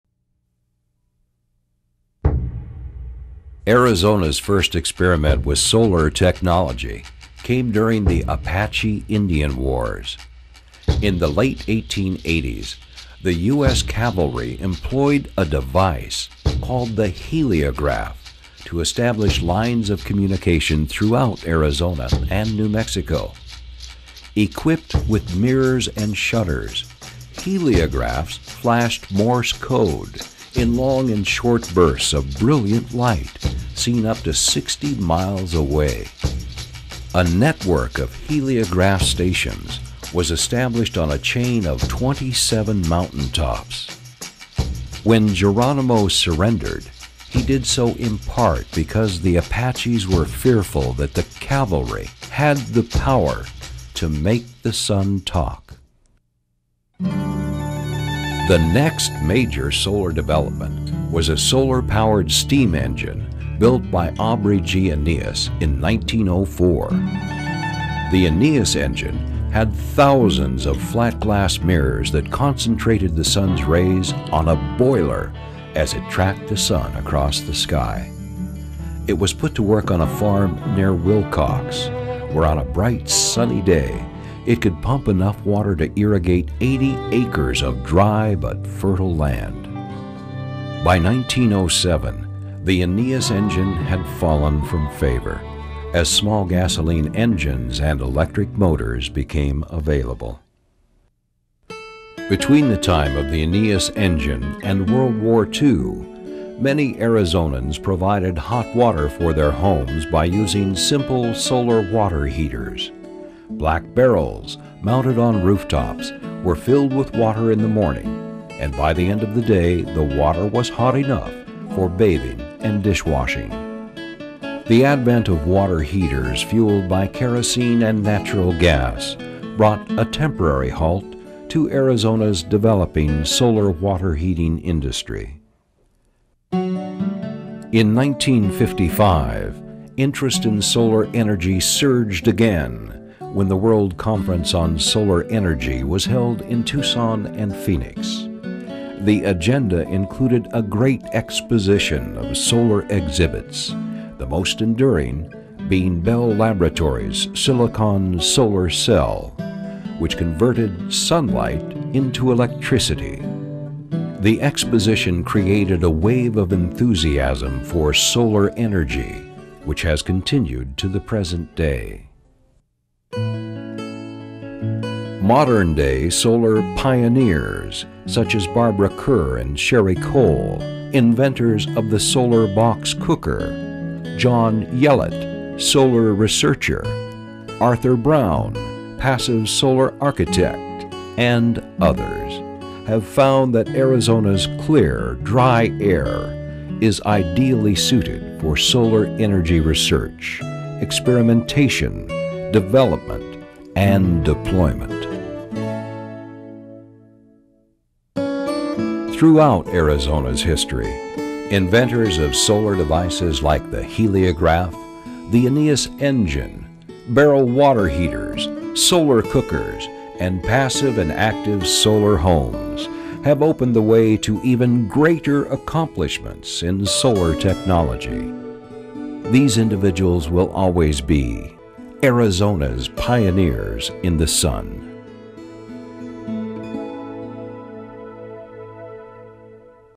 Pioneers of the Sun
This video examines early efforts to utilize heliograph communications and solar energy in the state of Arizona and includes reenactments of their original uses. The heliograph was one of the first solar experiments in territorial Arizona. In the late 1880s, the U.S. Cavalry employed the heliograph during hostilities with native Americans. Solar developments in Arizona during early part of the 20th century are also explored, from the Eneas Engine and the Day & Night solar water heater to solar cookers and passive solar architecture. Pioneers Arthur Brown, John Yellot, Sherry Cole and Barbara Kerr are recognized for their contributions to solar development in Arizona and elsewhere.